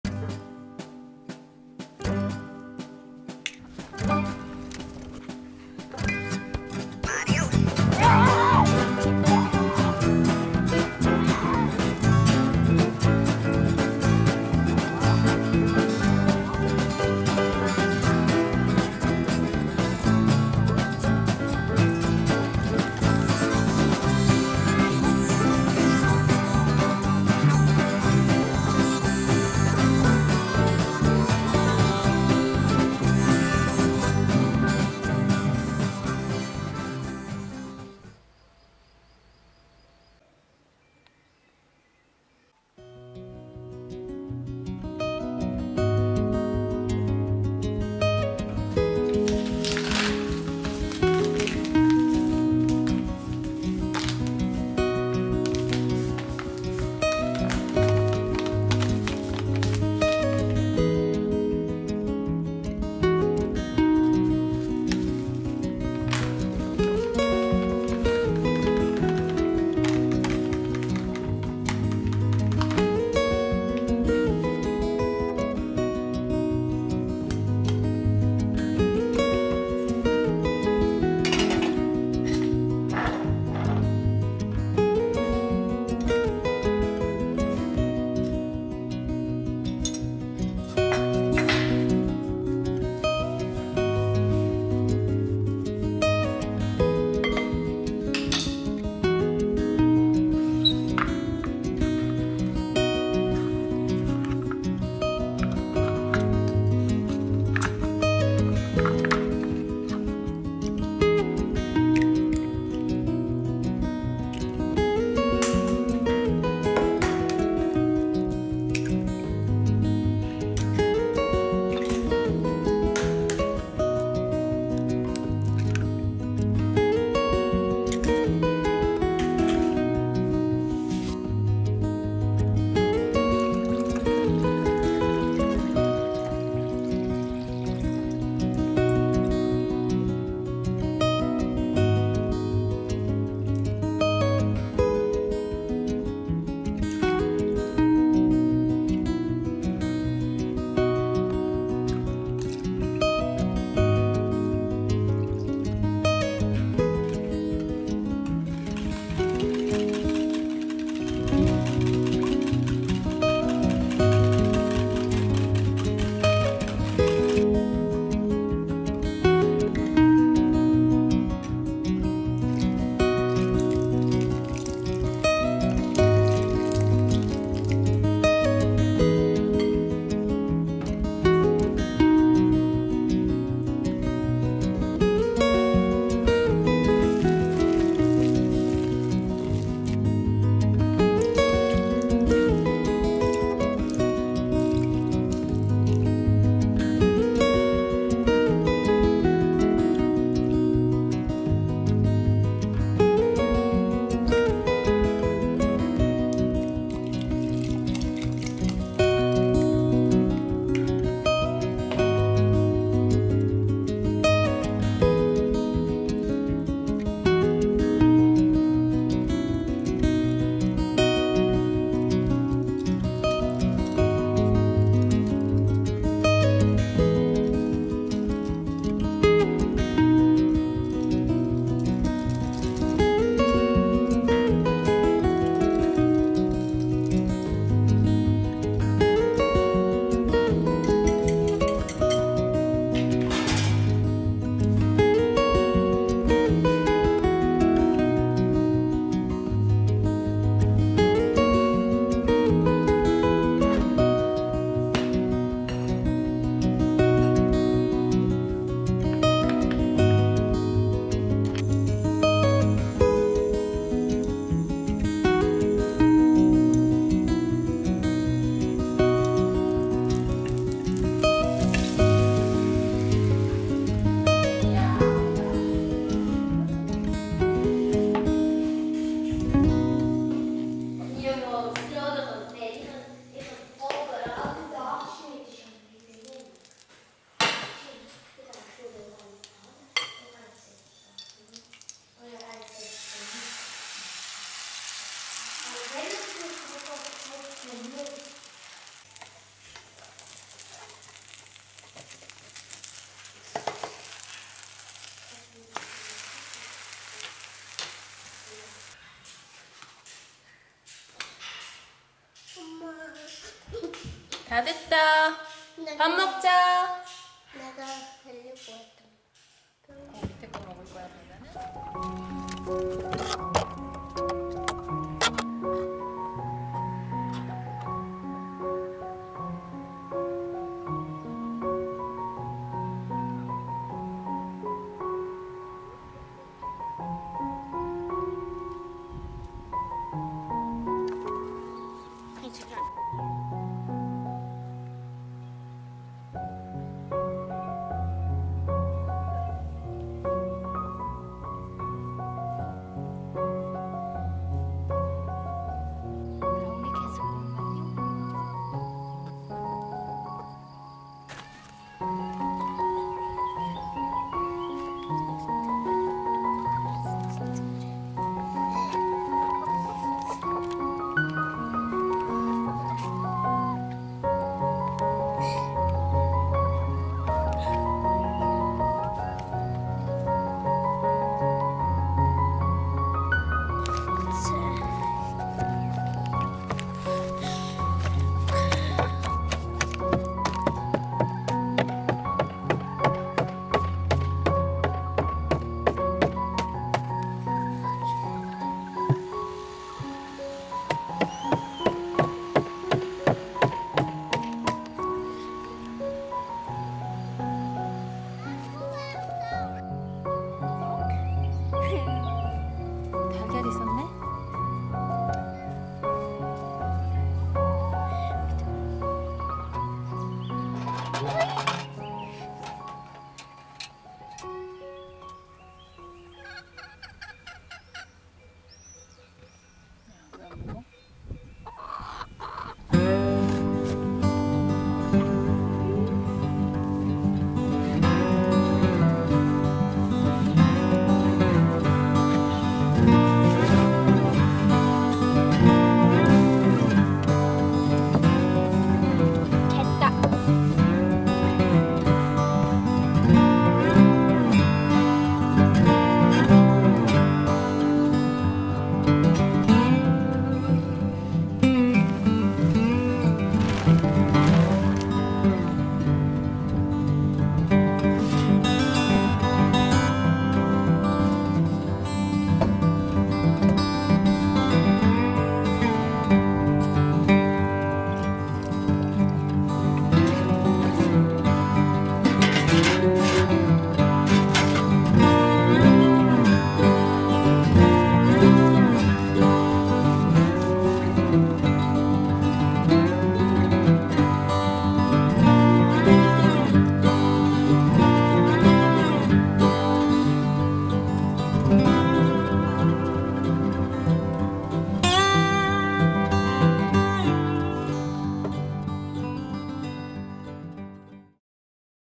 스위스식 오믈렛 만들기 /닭을 직접 키우는 어린이의 일상
오늘의 점심 메뉴는 스위스식 오믈렛입니다.

아이가 직접 키운 닭의 달걀로 만들어 먹으니 더 맛이 좋은 것 같아요.

동물을 돌본다는 것은 한 생명을 책임지고 보살펴야 하는 일이기에  아이에게 책임감을 심어줄 수 있는 최고의 교육인 것 같습니다.

아침마다 닭장 문을 열어주고, 먹이통과 물통을 채워주고, 정기적으로 닭장 청소도 해야하는 일이지만 아이는 싫은 내색 없이 누가 시키지 않아도 맡은 일을 꾸준히 합니다.

그렇게 아이가 자라고 있어요.

영상이 마음에 드셨다면 ‘구독’과 ‘알람설정’ 부탁드려요. 😊